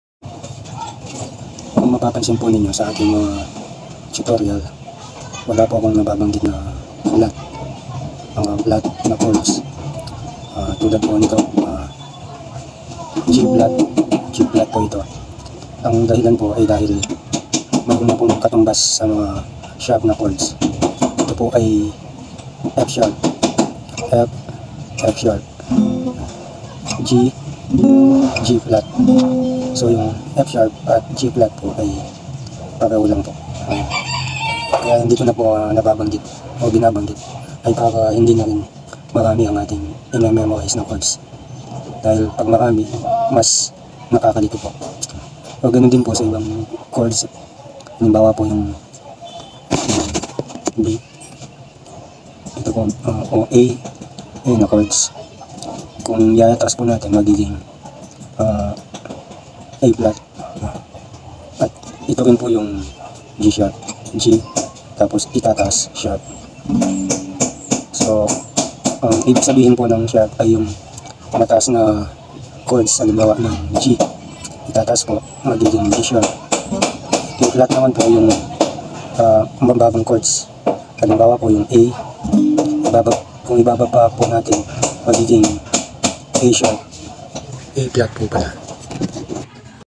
Flat and sharp chords.
Paliwanag sa flat at sharp chords. @Benscompositions